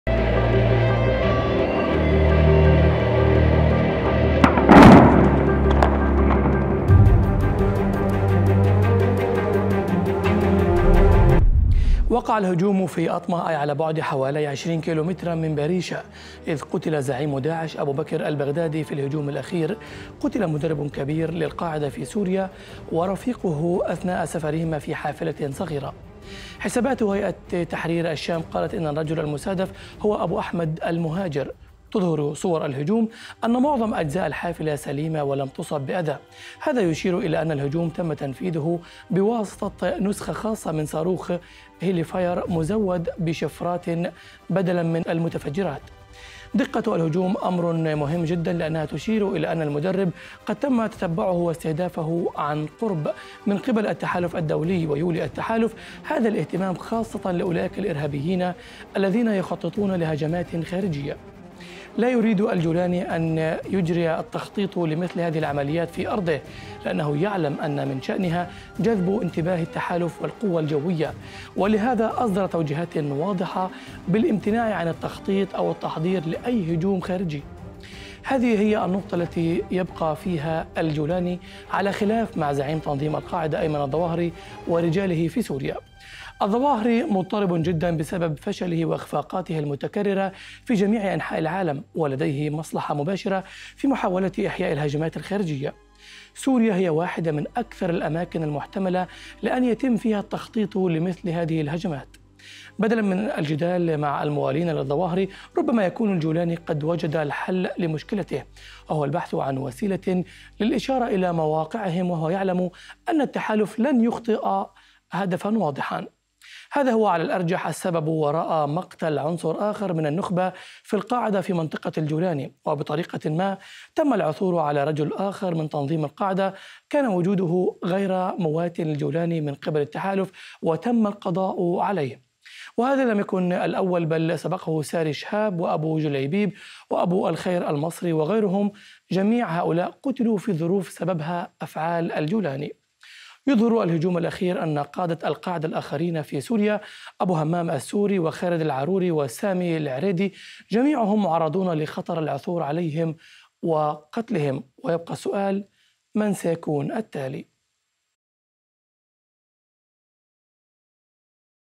في إدلب، من سيكون التالي من الموالين للظواهري في عمليات الاغتيال المرتبطة بالجولاني؟
أخبار الآن | إدلب - سوريا ( تحليل )

قَتلُ مدَربٍ من نخبة القاعدة في إدلب في غارة جوية للتحالف هو الأحدث في سلسلة العمليات الغامضة التي تحدث في الأراضي الواقعة تحت سيطرة هيئة تحرير الشام.

دقة الهجوم الذي وقع في 3 ديسمبر تثير تساؤلات حول الاقتتال الجهادي في شمال غرب سوريا.
وقع الهجوم في اطمه، أي على بعد حوالي 15 كيلومتر من باريشا حيث قتل زعيم داعش أبو بكر البغدادي. في الهجوم الأخير، قُتل مدرب كبير للقاعدة في سوريا ورفيقه أثناء سفرهما في حافلة صغيرة.

حسابات هيئة تحرير الشام قالت إن الرجل المستهدف هو أبو أحمد المهاجر. تظهر صور الهجوم أن معظم أجزاء الحافلة سليمة ولم تصب بأذى. هذا يشير إلى أن الهجوم تم تنفيذه بواسطة نسخة خاصة من صاروخ هيلفاير مزود بشفرات بدلاً من المتفجرات.

دقة الهجوم أمر مهم جدًا لأنها تشير إلى أن المدرب قد تم تتبعه واستهدفه عن قرب من قبل التحالف، ويولي التحالف هذا الاهتمام خاصةً لأولئك الإرهابيين الذين يخططون لهجمات خارجية.

لا يريد الجولاني أن يجري التخطيط لمثل هذه العمليات في أرضه، لأنه يعلم أن من شأنها جذب انتباه التحالف والقوة الجوية، ولهذا أصدر توجيهات واضحة بالامتناع عن التخطيط أو التحضير لأي هجوم خارجي.

هذه هي النقطة التي يبقى فيها الجولاني على خلاف مع زعيم تنظيم القاعدة أيمن الظواهري ورجاله في سوريا.

الظواهري مضطرب جدًا بسبب فشله وإخفاقاته المتكررة في جميع أنحاء العالم، ولديه مصلحة مباشرة في محاولة إحياء الهجمات الخارجية. سوريا هي واحدة من أكثر الأماكن المحتملة لأن يتم فيها التخطيط لمثل هذه الهجمات.

بدلاً من الجدال مع الموالين للظواهري، ربما يكون الجولاني قد وجد الحل لمشكلته، وهو البحث عن وسيلة للإشارة إلى مواقعهم، وهو يعلم أن التحالف لن يخطئ هدفًا واضحًا.

باختصار، هناك عنصر آخر من النخبة في القاعدة قتل في منطقة الجولاني. وبطريقة ما، تم العثور على رجل آخر من تنظيم القاعدة كان وجوده غير مواتٍ للجولاني من قِبل التحالف وتم القضاء عليه.

وهذا لم يكن الأول، بل سبقه ساري شهاب وأبو جليبيب وأبو الخير المصري وغيرهم، جميع هؤلاء قتلوا في ظروف تُعزى إلى أفعال الجولاني أو امتناعه عما يجب فعله.

يظهر الهجوم الأخير أن قادة القاعدة الآخرين في سوريا، أبو هُمام السوري وخالد العروري وسامي العريدي، جميعهم معرضون لخطر العثور عليهم وقتلهم. ويبقى السؤال، من سيكون التالي؟